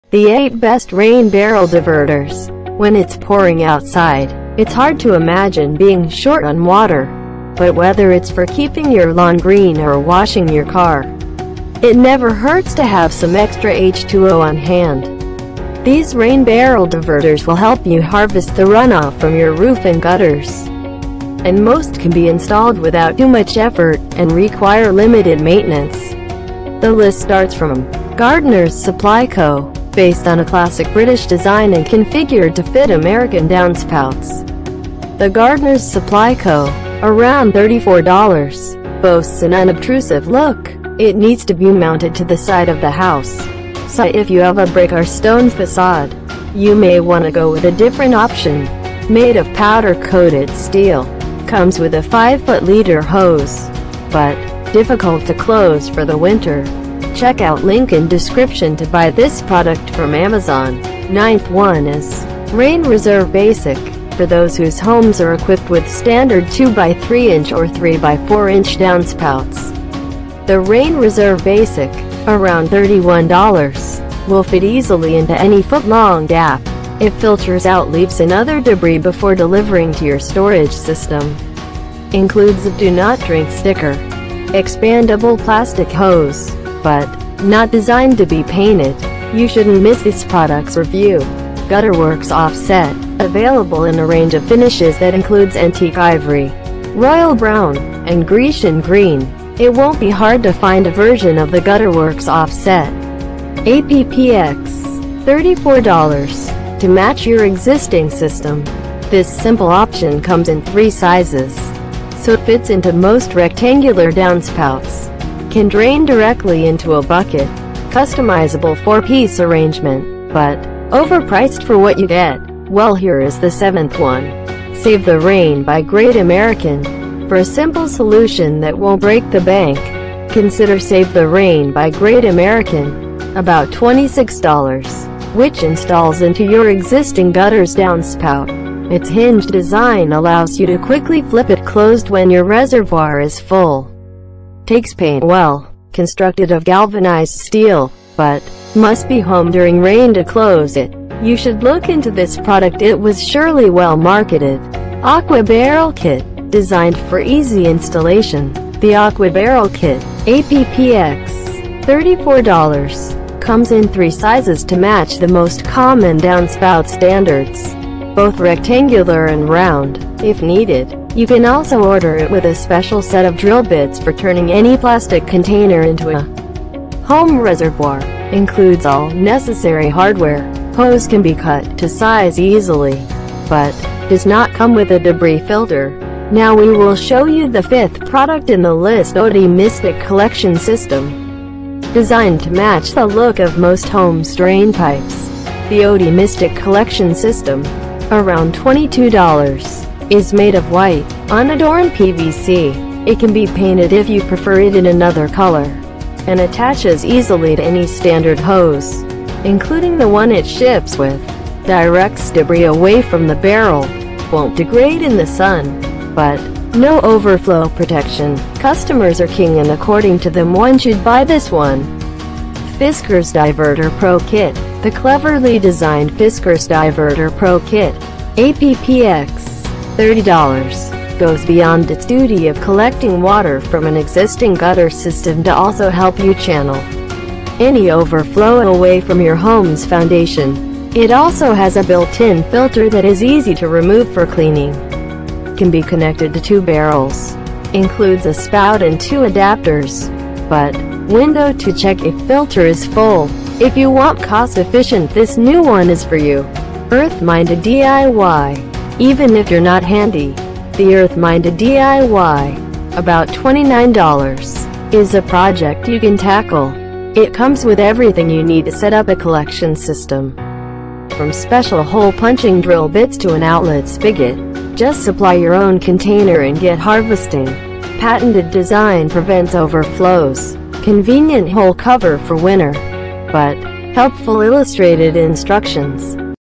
👉 The 8 Best Rain Barrel Diverters 2020 (Review Guide)
After 100's of customers and editors reviews of Best Rain Barrel Diverters, we have finalised these Best 8 products:

👉 1 Earth Minded DIY
👉 2 Fiskars DiverterPro Kit
👉 3 Oatey Mystic Collection System
👉 4 Aquabarrel Kit
👉 5 Save the Rain by Great American
👉 6 Gutterworks Offset
👉 7 Rain Reserve Basic
👉 8 Gardener's Supply Co.